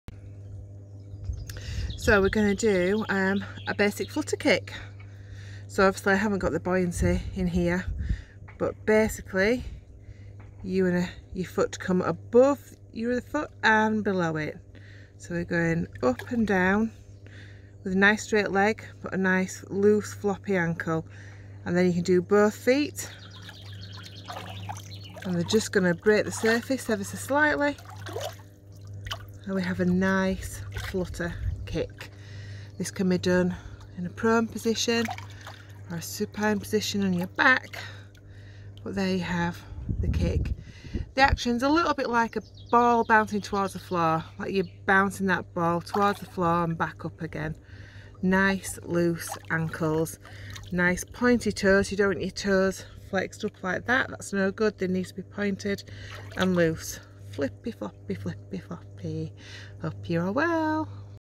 Flutter Kick, Home Swimming Practice.
Ways to practice a good flutter kick at home, in the bath, laid on the floor whatever works for you, but with some key points.